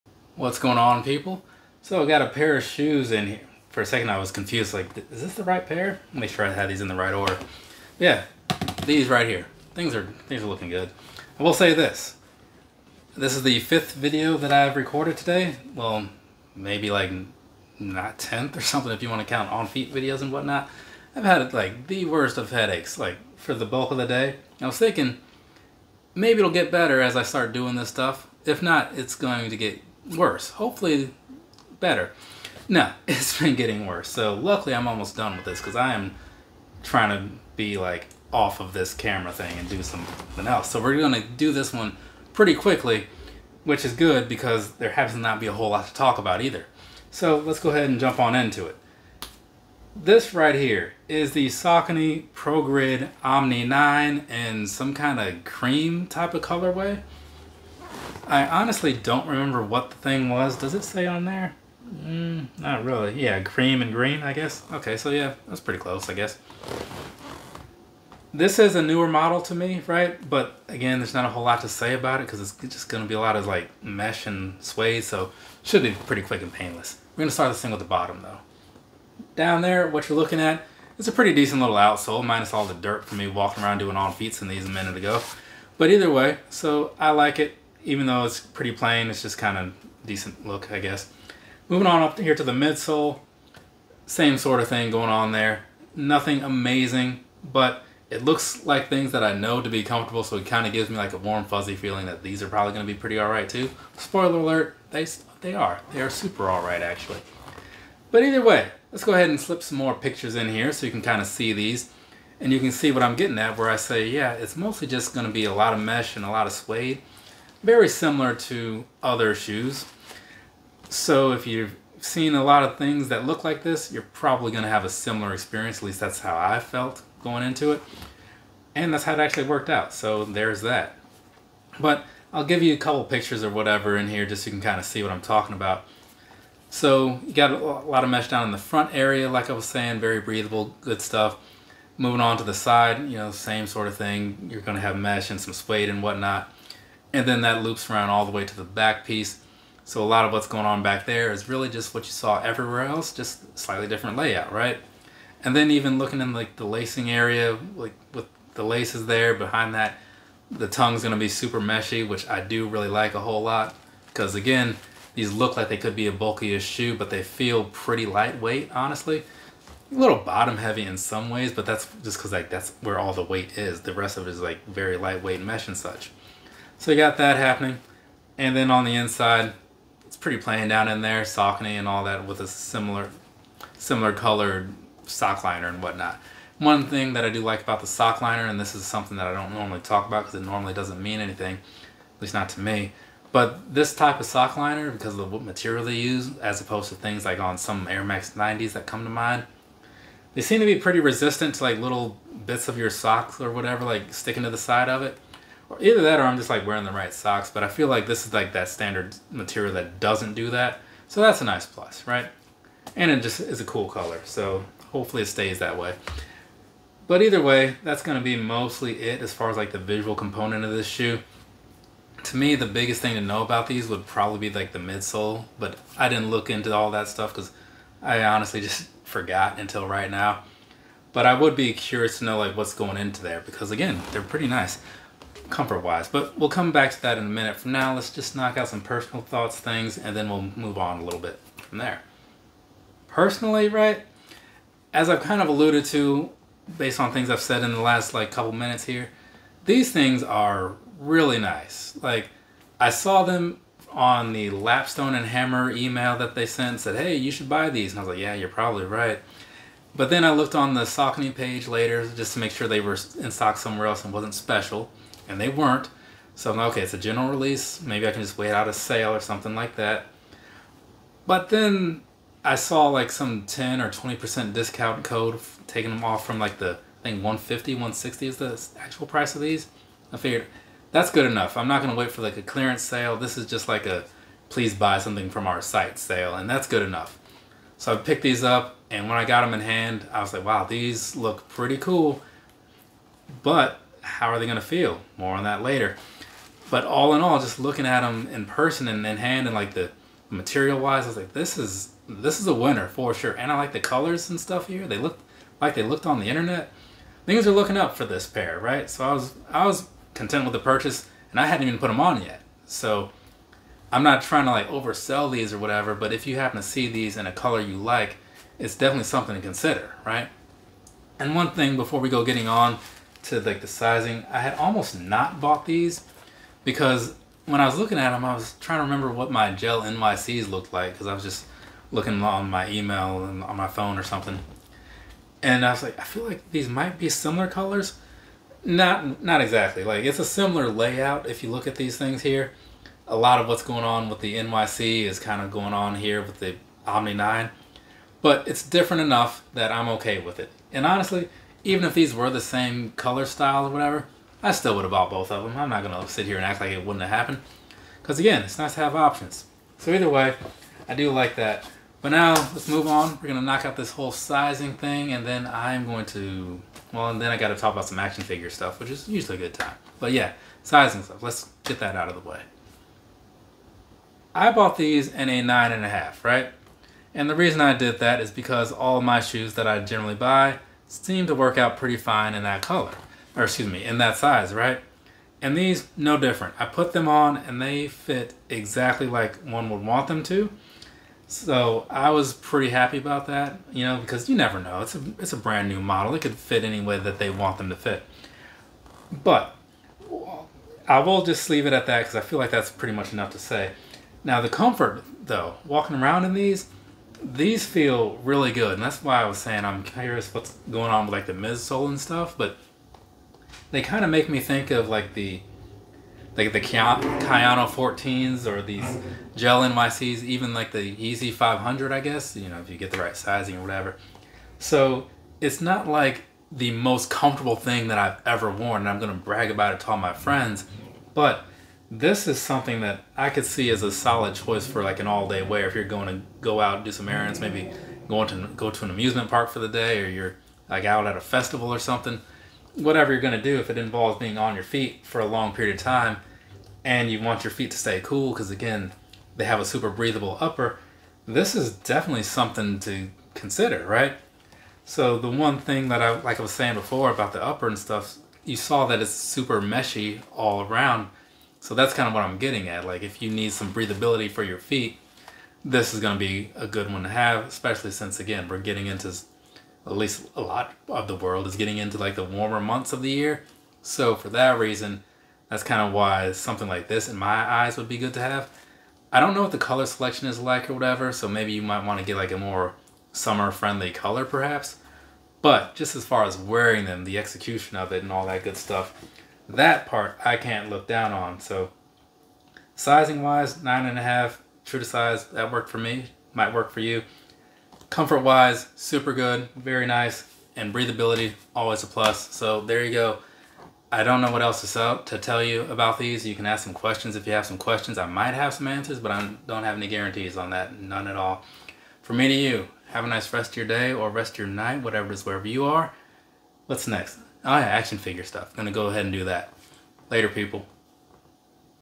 Saucony Progrid Omni 9 Premium Cream Green On Feet Review (S70740 13)
The Saucony ProGrid Omni 9 Premium grabbed my attention instantly. The model looked pretty good, but it was likely more so because of the Cream Green colorway (Style Code: S70740 13). Or maybe the combination. Whatever. It looked amazing and I wanted it. Not for full price, of course. But, regardless, I did want it. So I got it. Now I’m gonna tell you if this Cream Green ProGrid Omni 9 was worth it.

This is an almost full review. The pictures of the Cream Saucony ProGrid Omni 9 will mostly speak for themselves. Lots of suede. Lots of mesh. That’s pretty much it. So, don’t expect too much in this Saucony Progrid Omni 9 review. I do cover ProGrid Omni 9 sizing though, so that may be of help to someone. That’s done in conjunction with getting the Cream Green ProGrid Omni 9 Premium on feet. That way you have something to listen to while looking at my not-so-fancy footwork.

Enjoy this Saucony ProGrid Omni 9 Premium Cream Green On Feet Review.

0:00 What's going on, people?
0:45 Review
4:00 My Opinion
6:15 On Feet/Sizing

This 4K video was recorded with a Samsung Galaxy S23 Ultra and edited in Adobe Premiere by Iowa's greatest shoetuber.